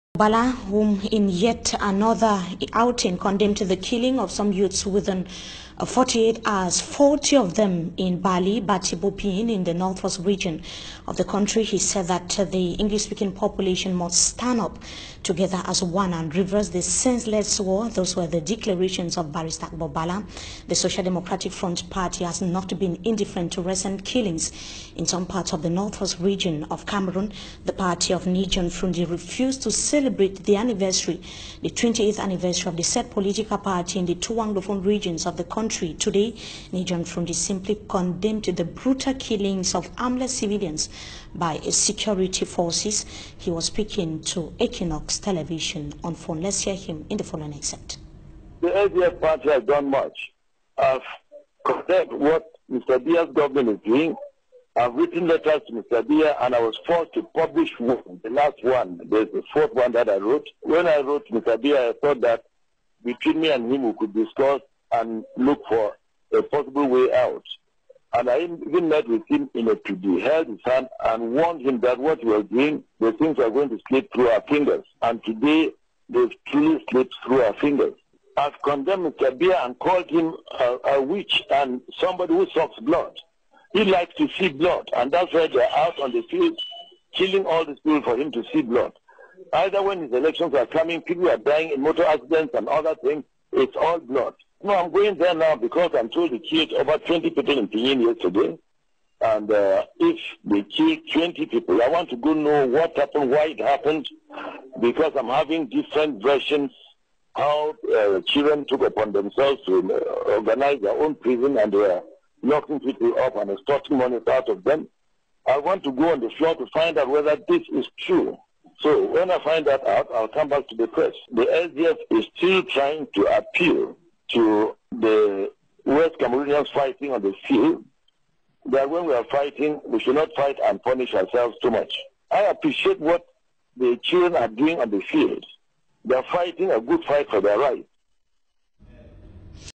Fru Ndi seem to have endorsed the Amba fighters! Watch & See for Yourself...
We Call for Inclusive Dialogue Between Anglophone & Francophone Region of Cameroon!